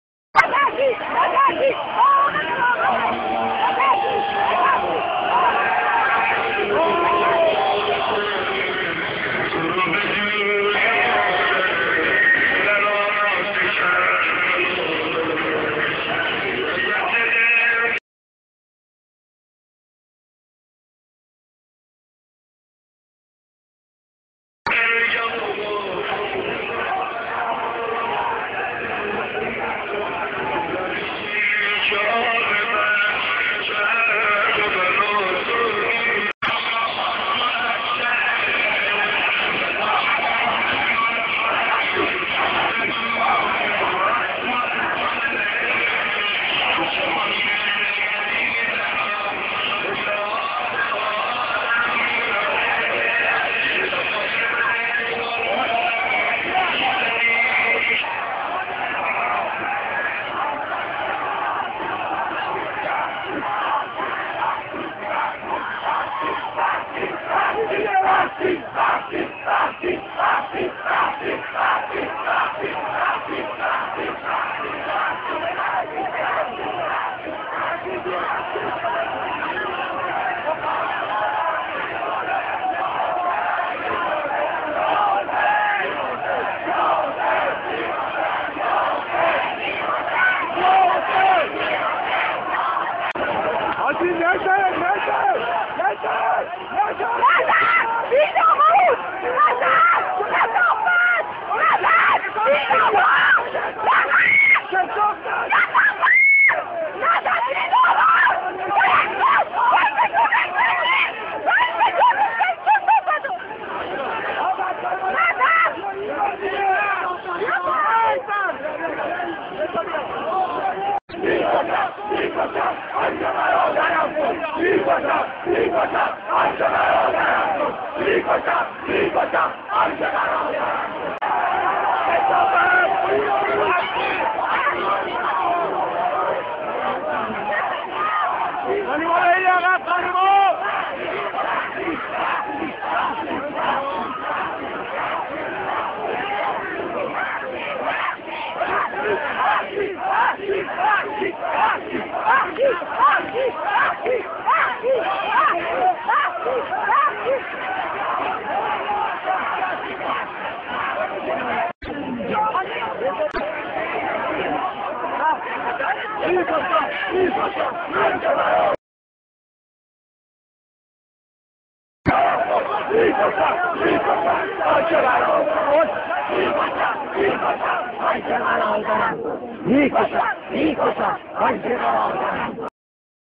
Basij is attacking students from Science & Tech with a knife Dec 30 2009
elm va sanat uni dec 29-چاقو کشی و حمله بسیج مزدور به دانشجویان
چاقو كشي، لگد پراكني، پرتاب سنگ و هرگونه وحشيگري ديگري كه بسيجيان و انصار حزب الله انجام ميدهند تا ترس و وحشت ایجاد کنن